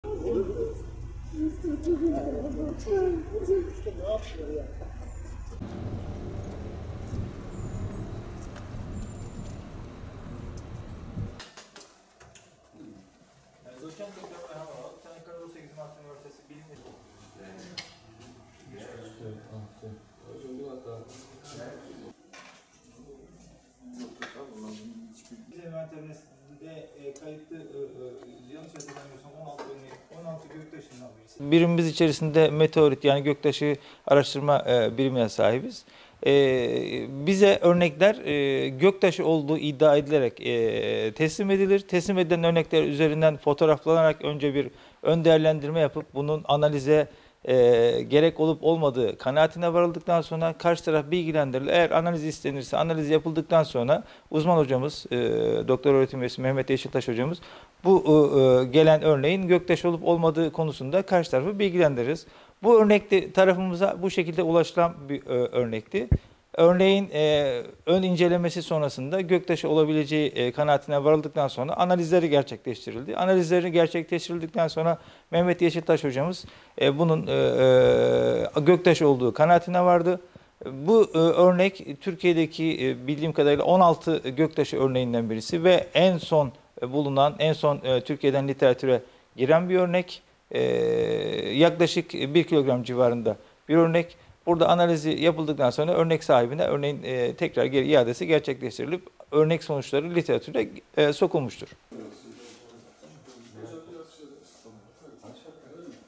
2008 Yılında Çiftçinin Bulduğu Taş "Göktaşı" Çıktı
Antalya’nın Kaş ilçesinde 2008 yılında çiftçi Mehmet Güngör’ün bahçesine düşen meteoriti (gök taşı), 11 yıl sonra ceviz ağacının dibinde bulunarak inceleme yapılması için Çanakkale Onsekiz Mart Üniversitesine (ÇOMÜ) gönderildi. Yapılan araştırmalarda taşın göktaşı olduğu kesinleşti.

Kaş ilçesinde yaşayan 81 yaşındaki Mehmet Güngör, 2008 yılında bahçesine düşen gök taşını 11 yıl sonra şans eseri bir ceviz ağacının dibinde buldu. Benzerlerine göre daha ağır ve demir içeriği yüksek olan taşın farklı bir tür olduğunu fark eden Güngör, inceleme yapılması için bu taşı Çanakkale Onsekiz Mart Üniversitesi (ÇOMÜ) Bilim ve Teknoloji Uygulama ve Araştırma Merkezine (ÇOBİLTUM) gönderdi. Merkezde, Kırıkkale Üniversitesi Dr. Öğr. Üyesi Mehmet Yeşiltaş tarafından yaklaşık 1 aydır süren incelemeler sonrası taşın gök taşı olduğu tespit edildi. Ender görülen gök taşının şu an büyük bir parçası çiftçi Mehmet Güngör’de, küçük bir kısmı da Dr. Öğr. Üyesi Mehmet Yeşiltaş'ta bulunuyor.

Ağırlığı yaklaşık 1 kilogram
Merkezde gök taşı ile ilgili yapılan araştırma hakkında bilgiler veren ÇOBİLTUM Müdürü Doç. Dr.Ayhan Oral, taşın ağırlığının yaklaşık 1 kilogram olduğunu belirterek, “Birimimiz içerisinde meteroit, yani gök taşı araştırma birimine sahibiz. Bize örnekler gök taşı olduğu iddia edilerek teslim edilir. Teslim edilen örnekler üzerinden fotoğraflandırılarak önce bir ön değerlendirme yapılıp tahlile gerek olup olmadığı kanaatine varırız. Daha sonra karşı taraf bilgilendirilir ve analiz yapıldıktan sonra alanında uzman hocamız Doktor Öğretim Üyesi Mehmet Yeşiltaş tarafından bu gelen örneğin gök taşı olup olmadığı tespit edilir. Bu örnek tarafımıza bu şekilde ulaştırıldı. Örneğin ön incelemesi sonrasında gök taşı olabileceği kanaatine varıldı ve sonra tahliller yapıldı. Mehmet Yeşiltaş hocamız bunun gök taşı olduğu kanaatine vardı. Bu örnek Türkiye’de bildiğim kadarıyla 16 gök taşı örneğinden birisi ve en son Türkiye’den literatüre giren bir örnek. Yaklaşık 1 kilogram civarında bir örnek. Burada analizi yapıldıktan sonra sahibine iade edilecek ve örnek sonuçları literatüre sokulmuş olacak” ifadelerini kullandı.